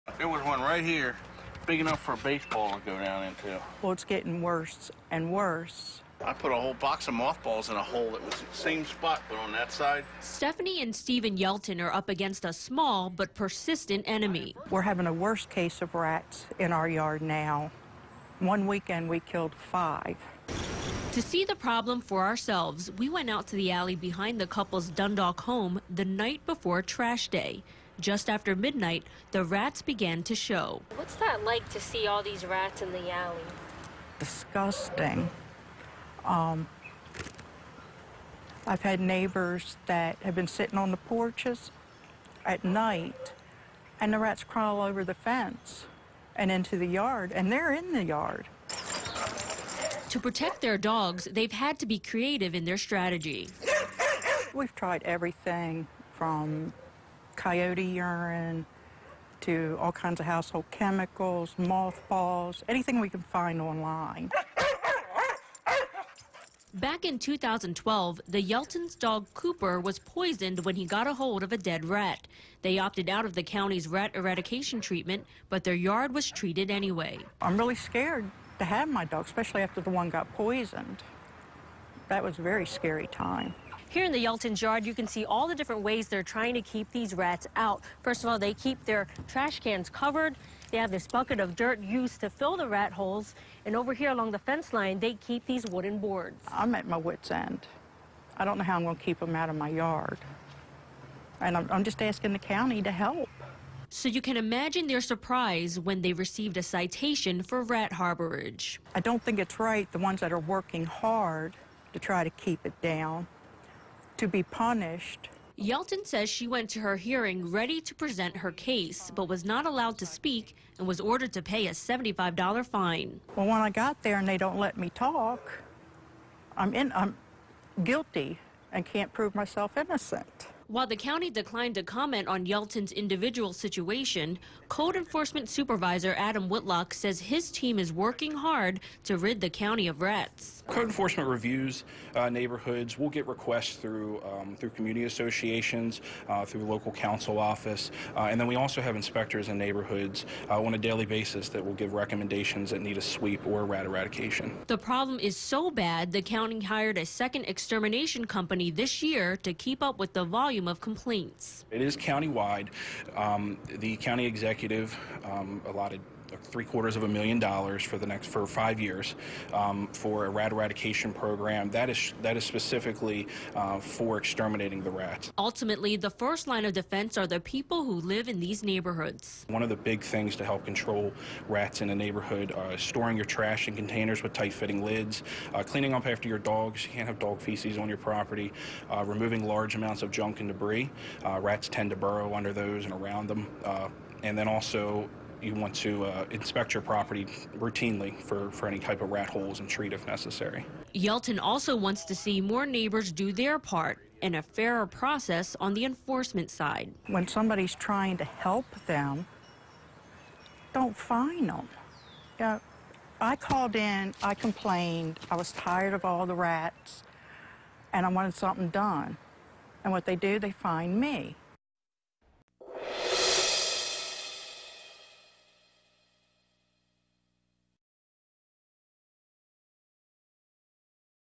Rat infestation in Dundalk prompting frustration, fines for homeowners
They're pests that carry diseases, multiply quickly and are hard to catch. Rodents are terrorizing a Baltimore County community. Stephanie Yelton of Dundalk called the county to report the problem--and ended up being fined.